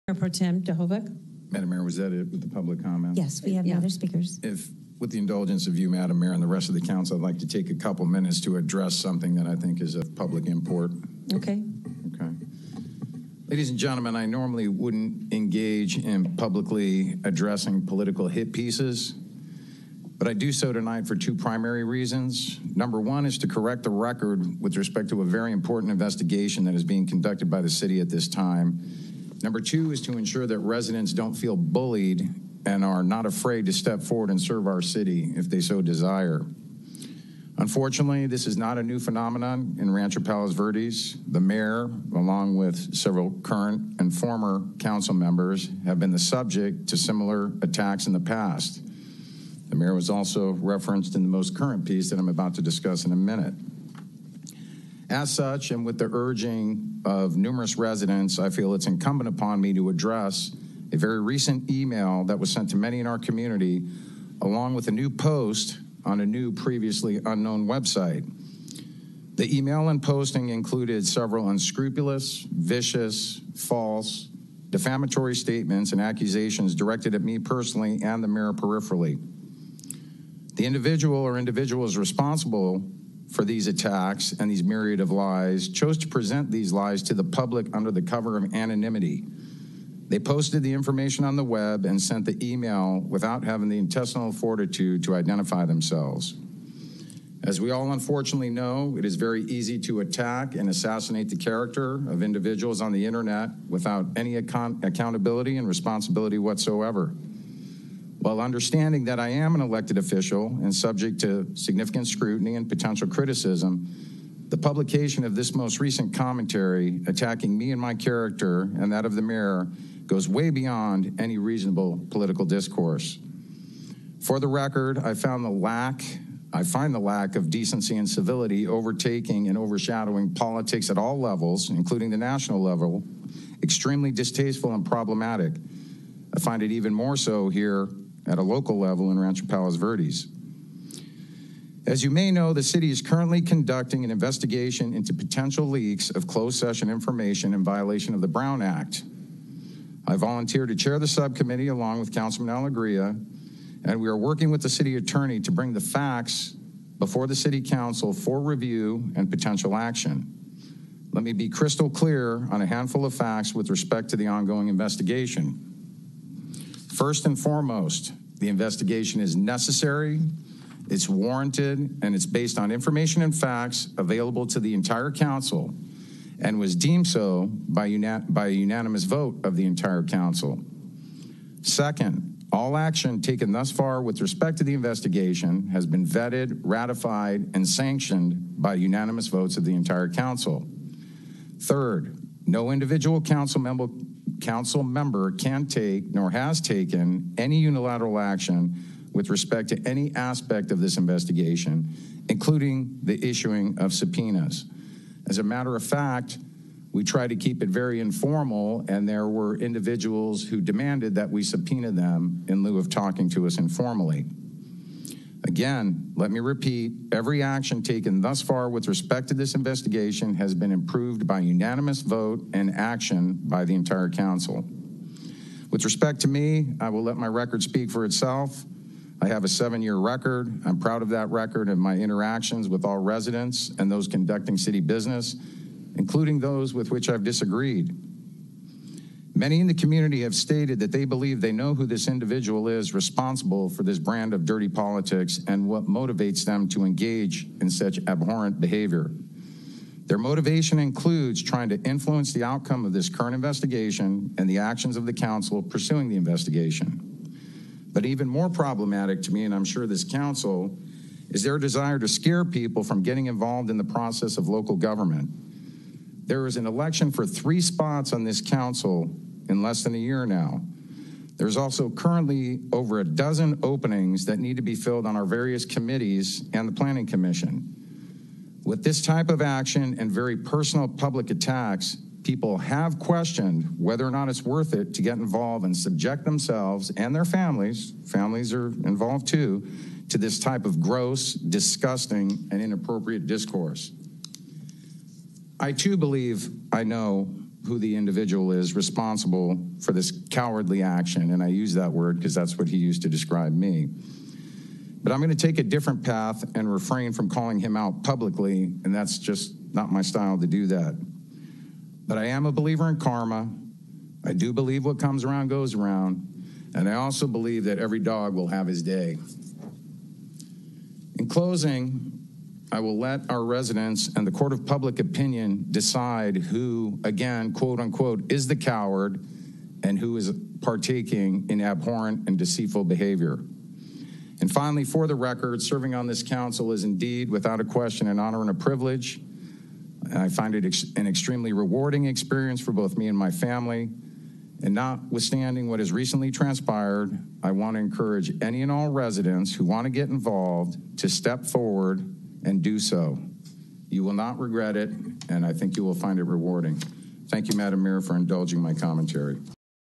Jerry Duhovic Comment - November 7th 2018
Rancho Palos Verdes Mayor Pro Tem Jerry Duhovic's comments from City Council Meeting on November 7th, 2018